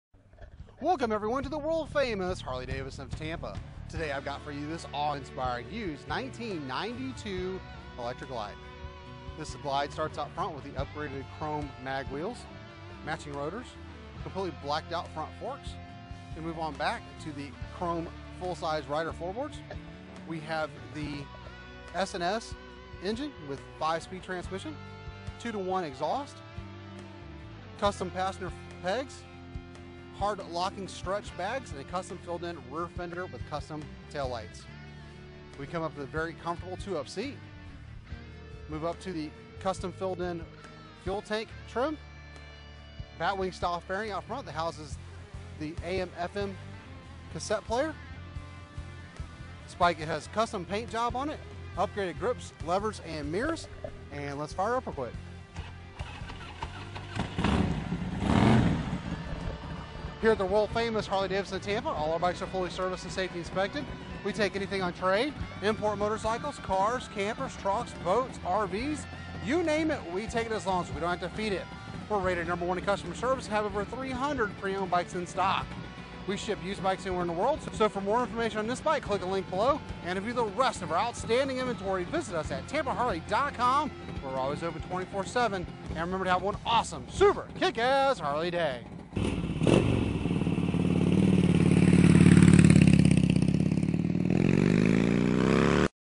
Used 1992 Harley Davidson Electra Glide Bikes for sale
Used 1992 Harley Davidson FLHT Electra Glide bikes for sale.

Featuring: chrome mag wheels, extended bags, custom paint job, blacked out front end, custom filled in rear fender, S&S engine, two to one performance exhaust

Stock# B1308B